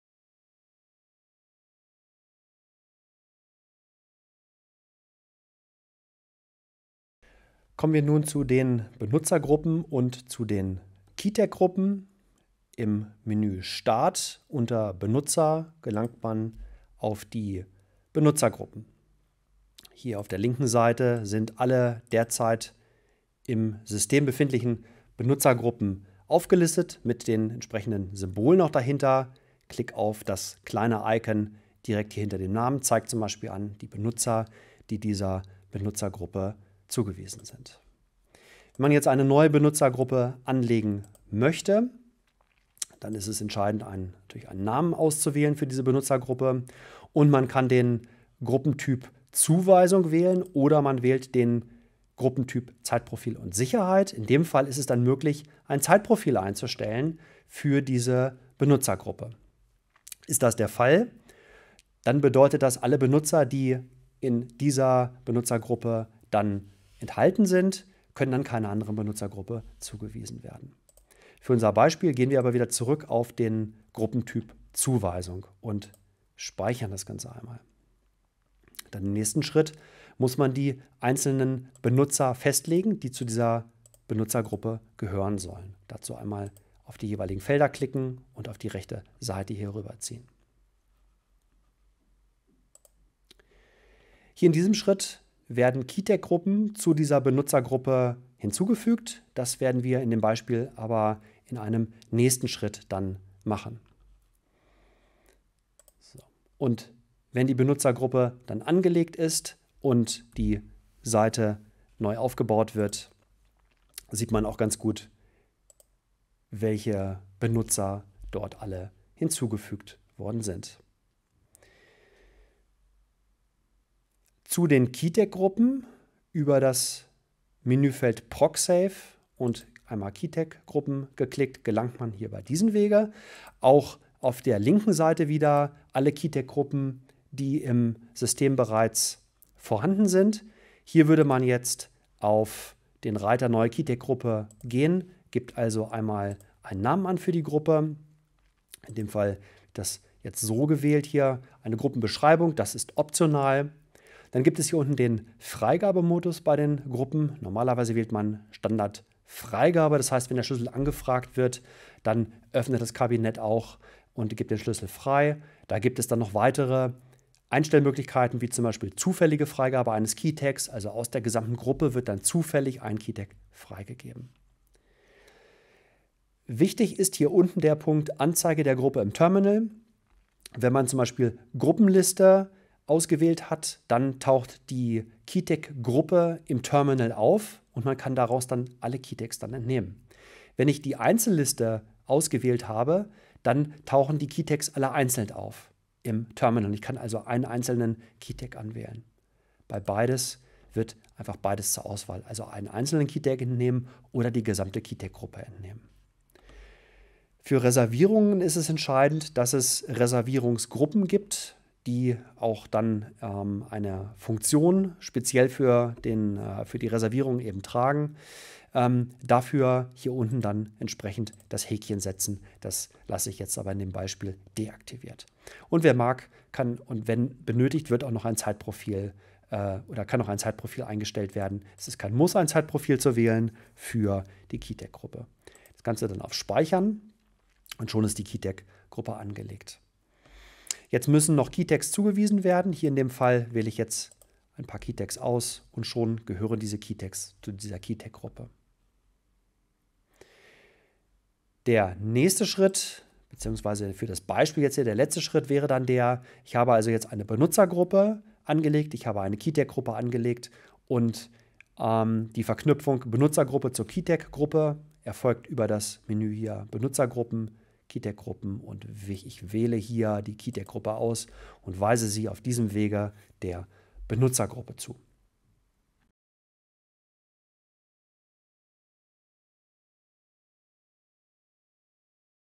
04 - Benutzergruppen und keyTag Gruppen | Commander 4.12.1 Tutorial
Commander 4.12.1 – Basics (Deutsch)

In diesem Tutorial beschäftigen wir uns mit den Benutzergruppen und keyTag-Gruppen innerhalb der Commander Software.

Möchten Sie mehr wissen?